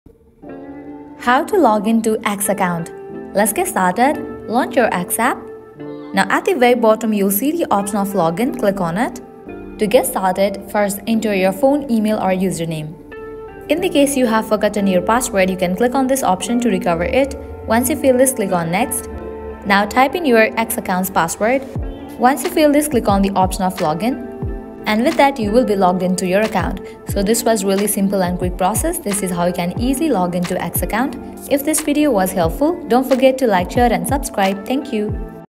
How To Login To X Or Twitter Account
In this tutorial, we'll provide you with a step-by-step guide on how to login to your X or Twitter account smoothly, and we'll also cover some troubleshooting tips in case you encounter any login errors.

👋 Welcome to Glitch Sorcerer! In this video, we'll guide you through the process of logging in to your X or Twitter account. If you're facing any issues with logging in or accessing your accounts on these platforms, you've come to the right place.

📱 Timestamps:

0:00 - Introduction
0:05 - How to Login to Your X or Twitter Account